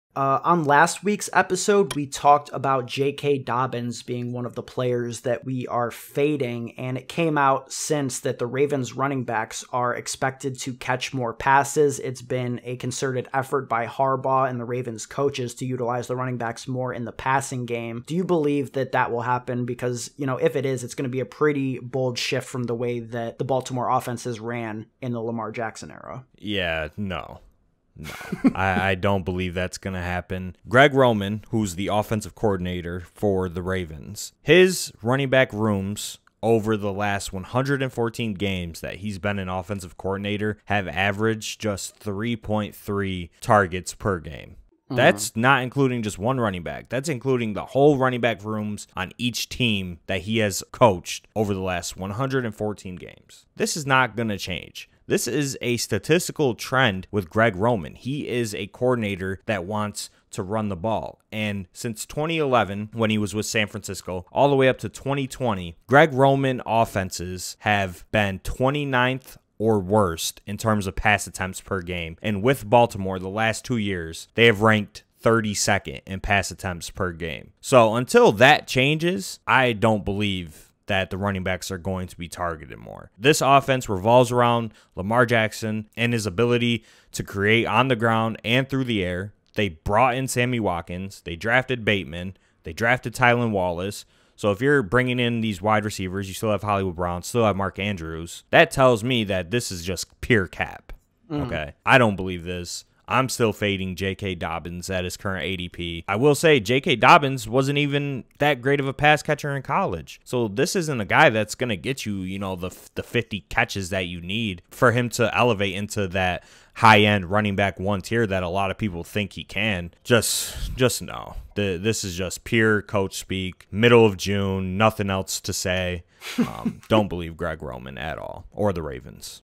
Fade J.K. Dobbins in Fantasy Football In 2021! - Fantasy Football Advice
In this clip, Ben and Joey discuss why you should be fading JK Dobbins in Fantasy Football in 2021.
JK Dobbins is going in the 3rd round in Best Ball Fantasy. Are you willing to pay that price tag for a player that isn't going to catch passes?

Do you agree or disagree? let us know down below in the comments?

You can find the LATEST EPISODE The DFS Dose Podcast on these platforms: